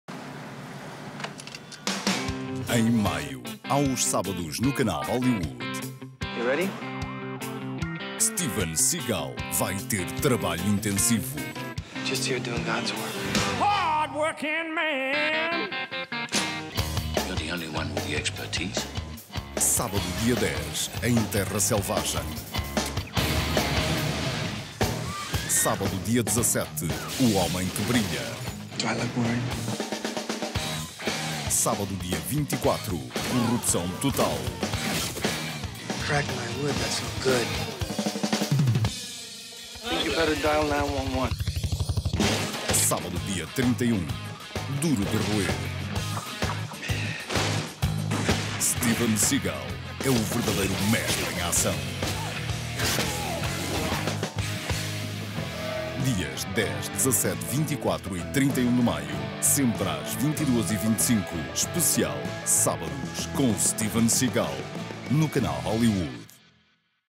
Sábados com Steven Seagal
Os sábados de 10, 17, 24 e 30 de maio prometem muita ação, luta e adrenalina, com o mestre de artes marciais Steven Seagal no papel principal.